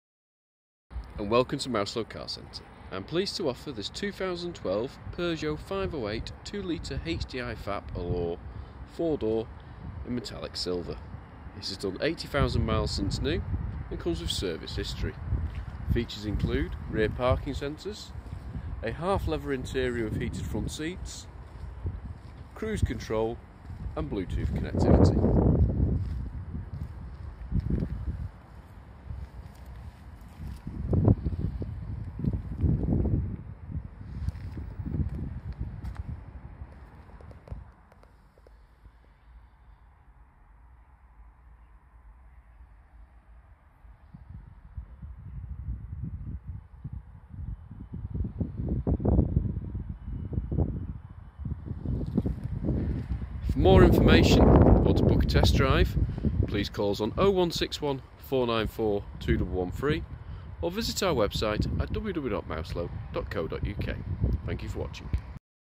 Peugeot 508 2.0 HDi FAP Allure 4dr
A fantastic spec PEUGEOT 508 in METALLIC SILVER complemented by 18” ALLOY WHEELS and FRONT FOG LIGHTS. Make your calls hands free using BLUETOOTH through the MULTIFUCTION STEERING WHEEL. Listen to all your favourite music through the integrated USB PORT. Safe for your children with REAR ISOFIX and TOP TEATHER ANCHORAGE POINTS. Drive in comfort whatever the weather with CRUISE CONTROL and DUAL CLIMATE CONTROL WITH AIR CONDITIONING. Park with ease using the rear PARKING SENSORS. Other features on this PEUGEOT 508 include a half LEATHER interior with HEATED FRONT SEATS, POWER FOLDING DOOR MIRRORS, ELECTRIC WINDOWS front and rear.